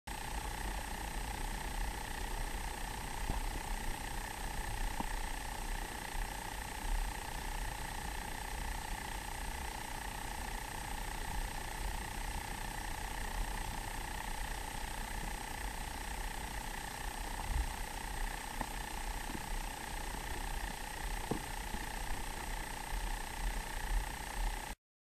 FISH SWALLOWS TADPOLE
A TADPOLE SWIMS INTO FISHES MOUTH AND GETS SPAT OUT